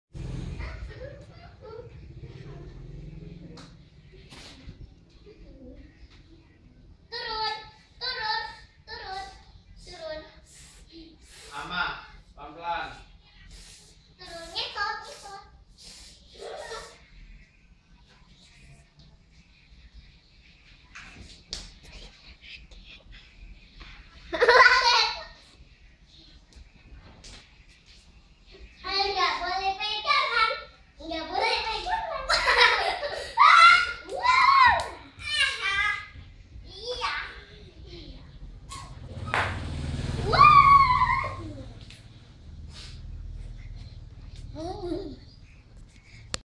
bermain di tangga NGAK BERANI TURUN
Azka akma turun tangga